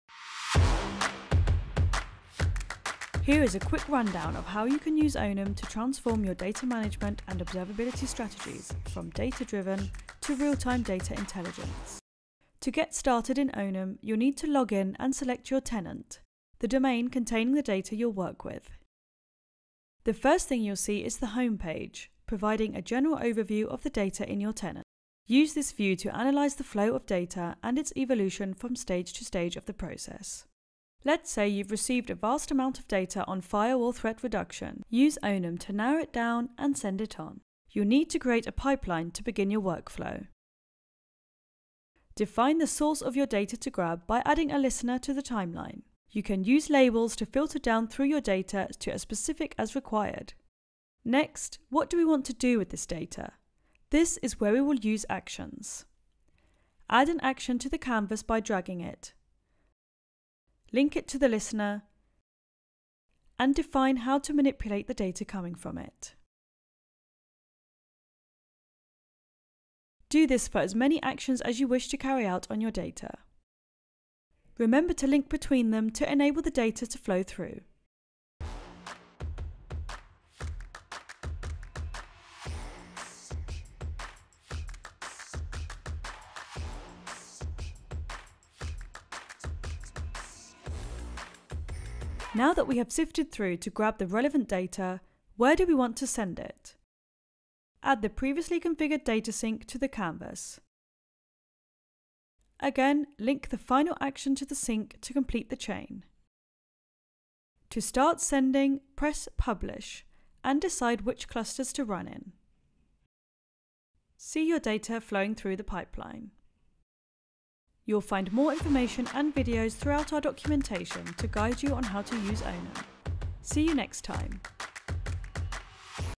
Get started with Onum
Here is a quick rundown of how you can use Onum to transform your data management and observability strategies from data-driven to real-time data intelligence.